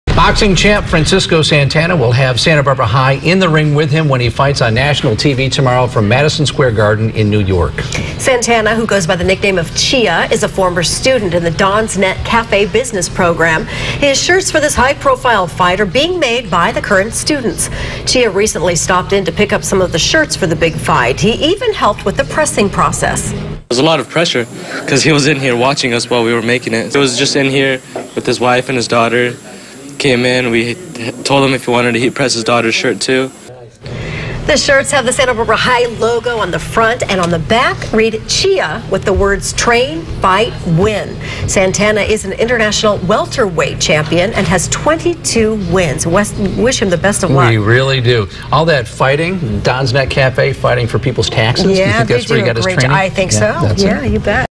FRANCISCO SANTANA T SHIRTS
BOXING CHAMP FRANCISCO SANTANA WILL HAVE SANTA BARBARA HIGH IN THE RING WITH HIM WHEN HE FIGHTS ON NATIONAL T.V. TOMORROW FROM MADISON SQUARE GARDEN IN NEW YORK.